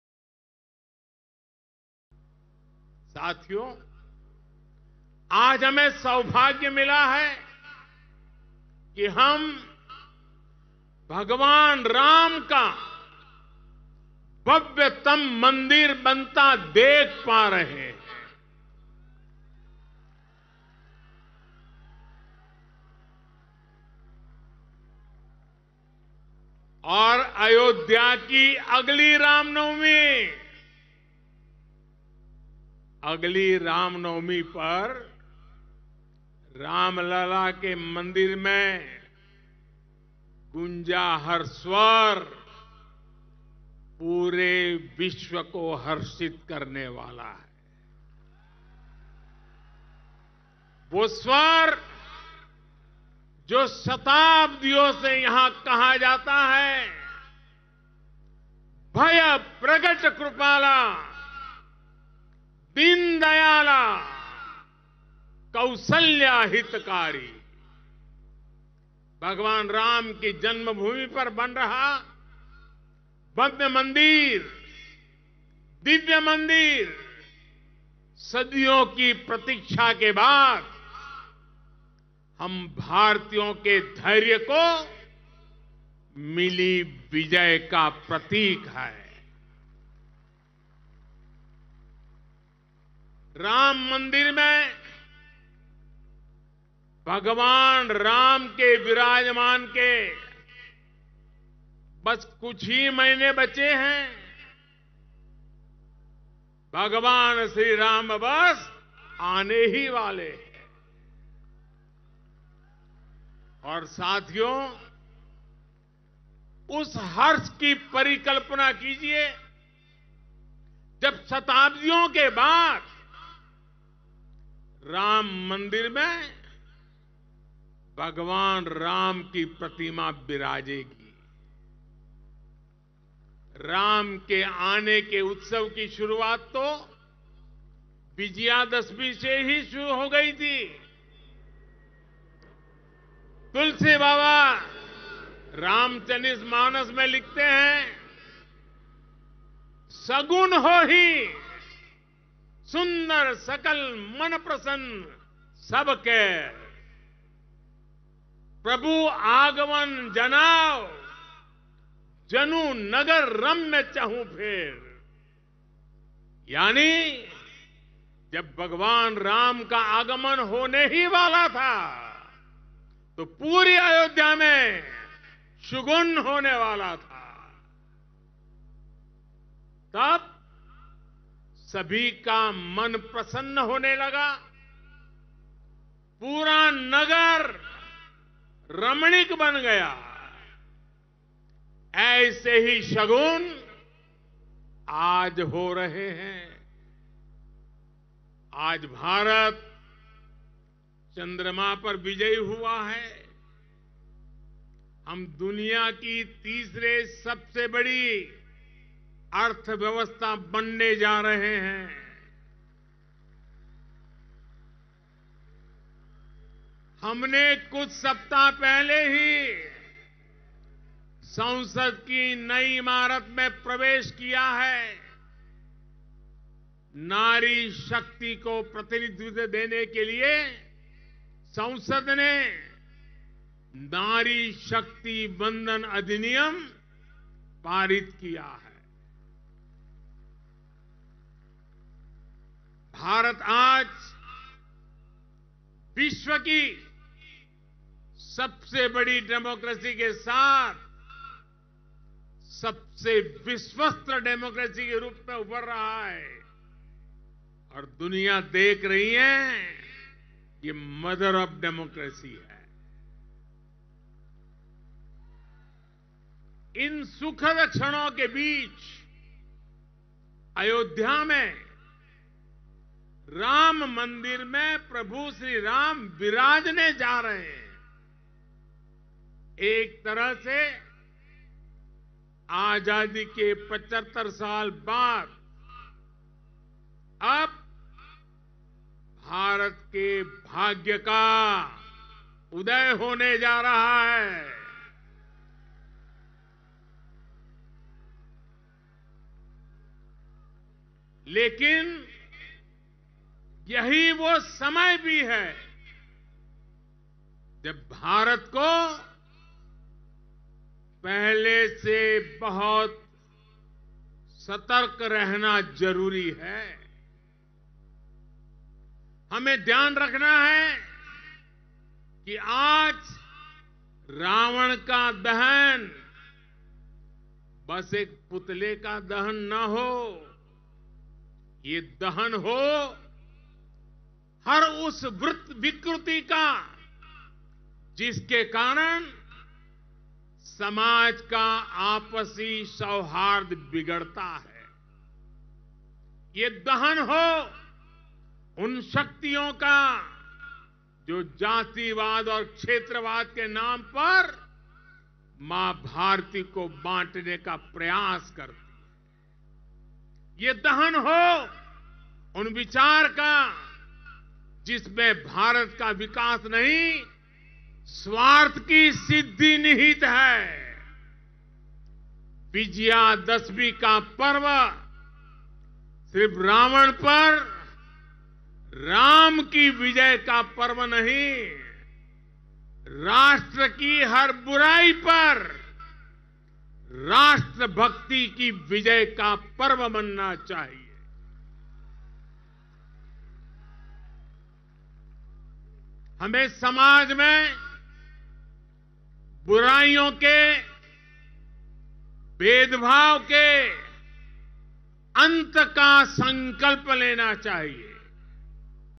ayodhya के राम मंदिर में भगवान श्री राम बस आने ही वाले हैं
The News India DT, Hindi language channel